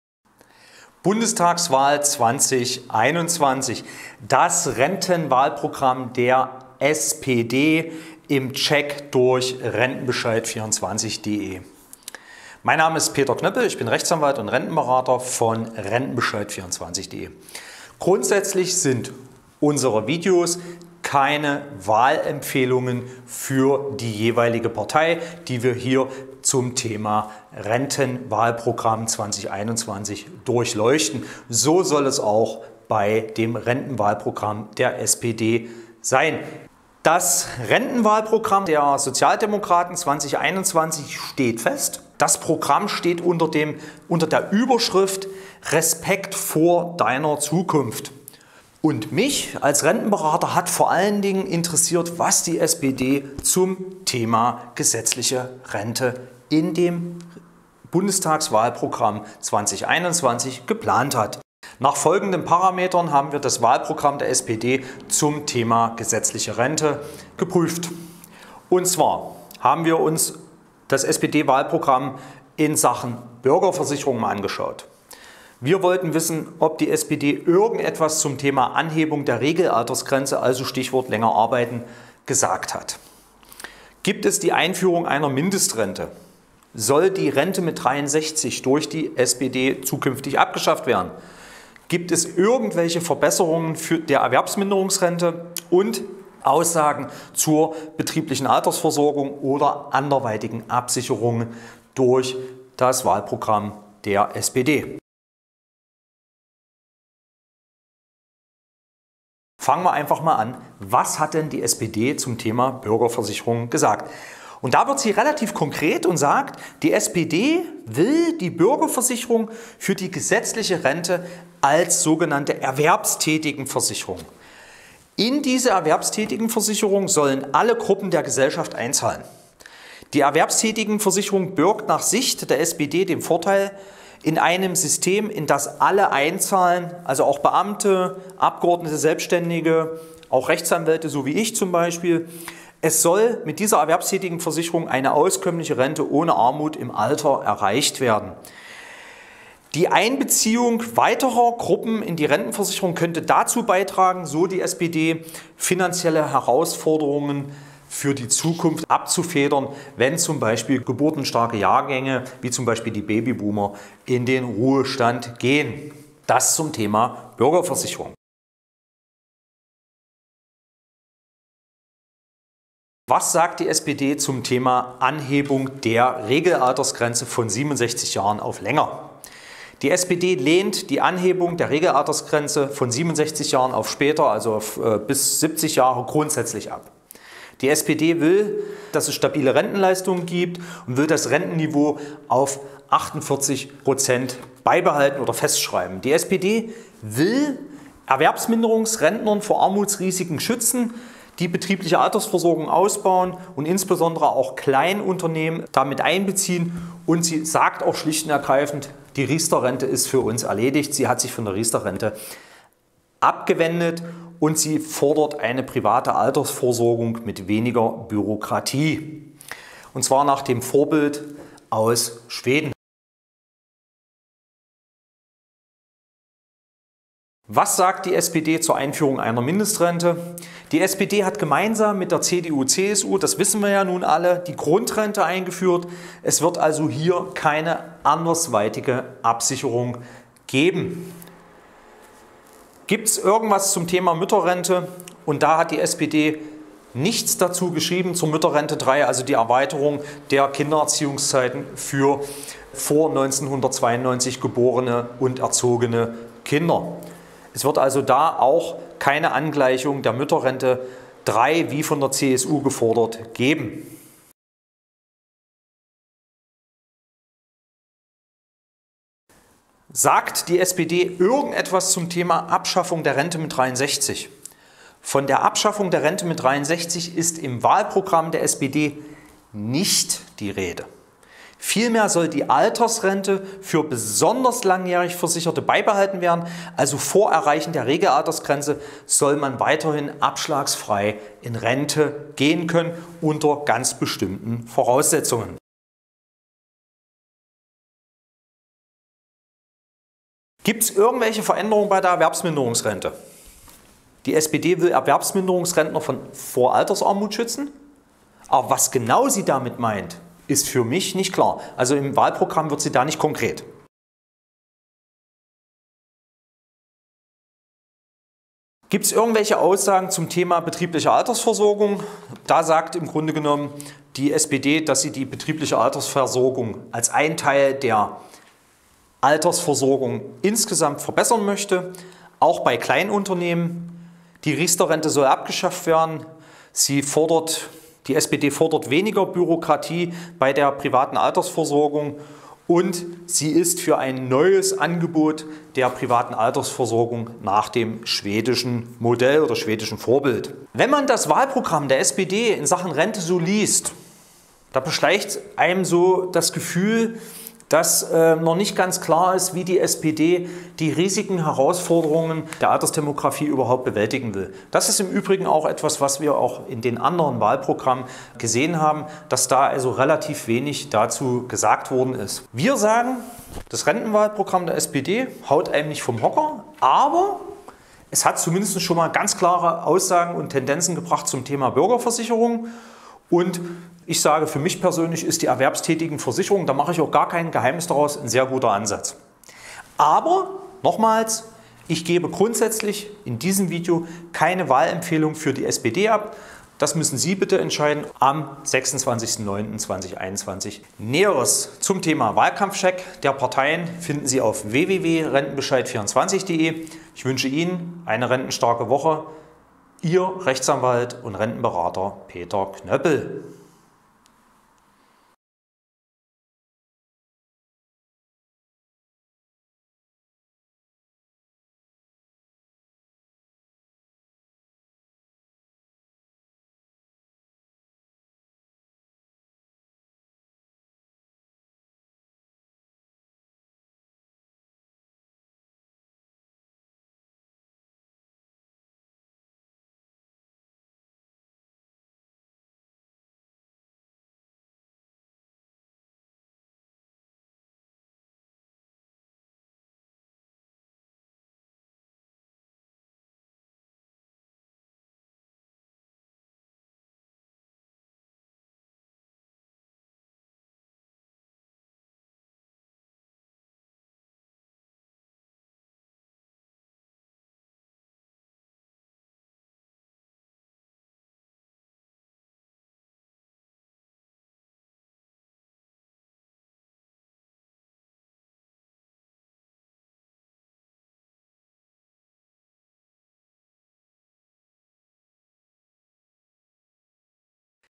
SPD Wahlprogramm - Fakten zur Rente - Bundestagswahl 2021
Auf  65 Seiten steht, was die SPD mit Deutschland für die Zukunft plant. Das Programm steht unter der Überschrift „Respekt vor Deiner Zukunft“. Als Rentenberater von rentenbescheid24.de interessiert uns vor allem, was in Sachen gesetzliche Rente durch die SPD geplant ist. Wir hatten in unserem Spezial Bundestagswahlkampf 2021 bestimmte Parameter festgelegt, die uns bei den Rentenprogrammen der Parteien für die Bundestagswahl vorrangig interessieren. Hier zum Nachlesen!

00:00 Vorstellung Wahlprogramm SPD
01:46 Bürgerversicherung
02:49 Regelaltersgrenze
03:44 Mindestrente
04:25 Rente 63
04:45 EM-Rente
05:15 Betriebsrente
05:50 Fazit Wahlprogramm SPD
06:55 Überblick